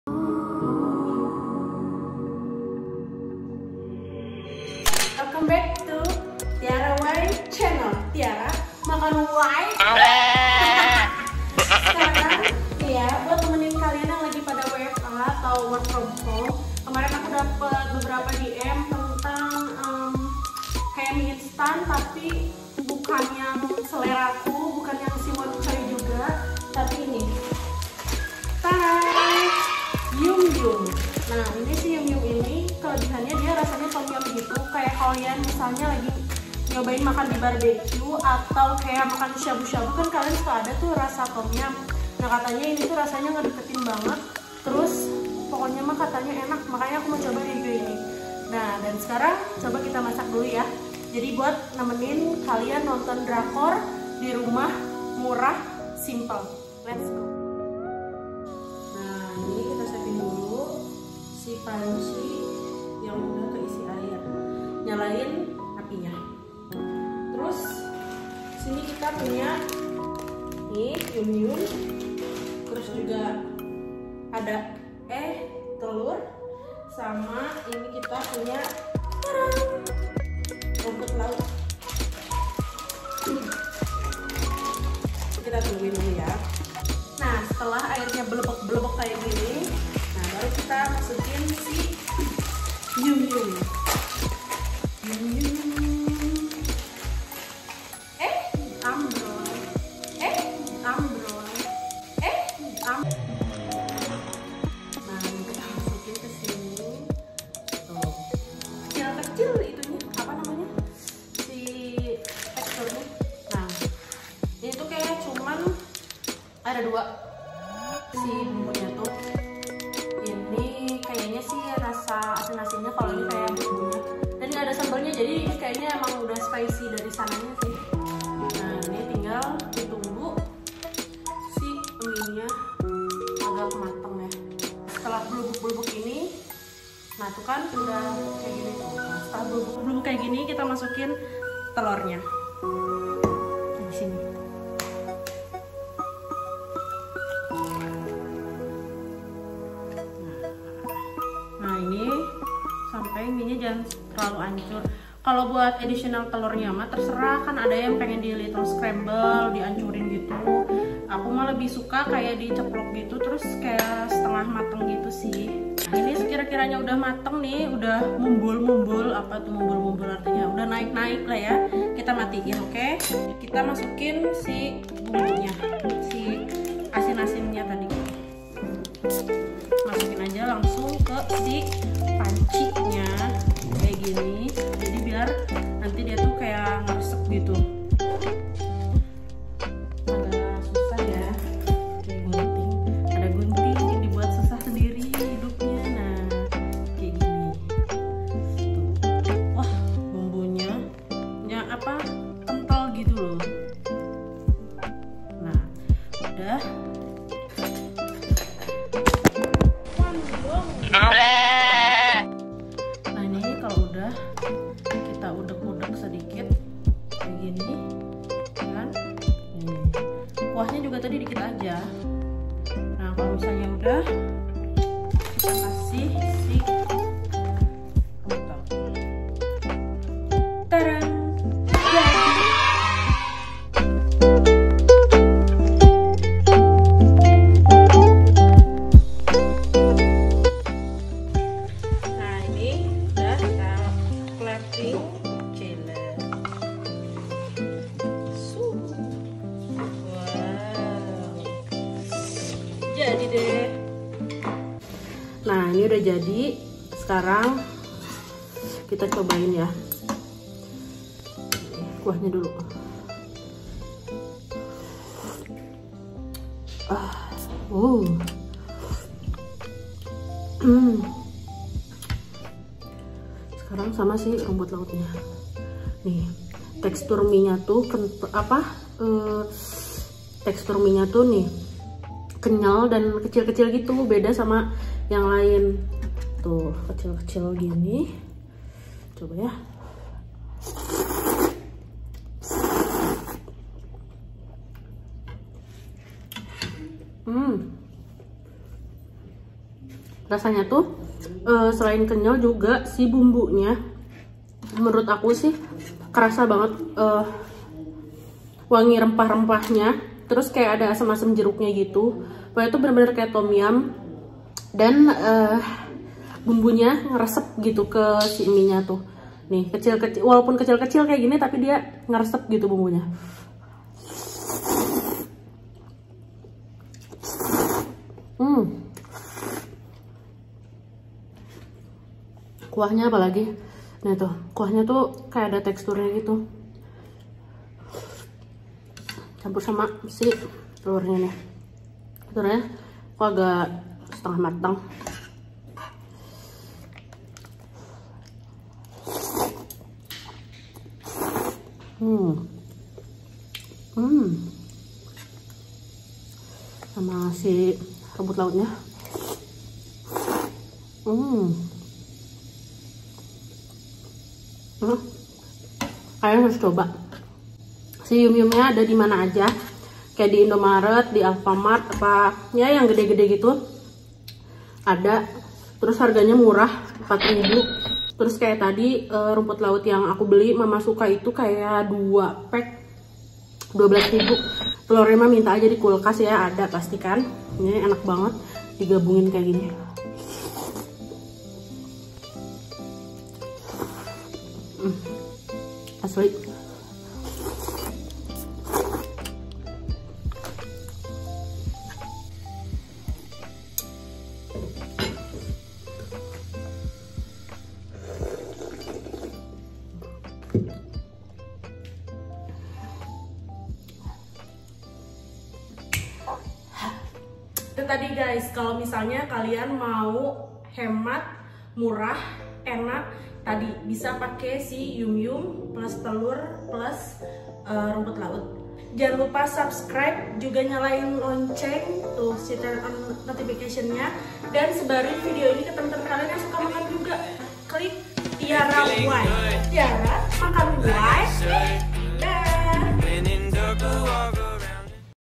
YUM YUM !! MIE MURAH RASA RESTORAN
Hai guys, udah lama nih Tiara ga upload video
Kali ini Tiara mau coba mie YUM YUM khas Thailand yang 100% Halal, gimana rasanya? Nonton yuk.
Buat kalian yang mau Tiara masak, nyobain atau review makanan apa, tulis di kolom komentar yaa guys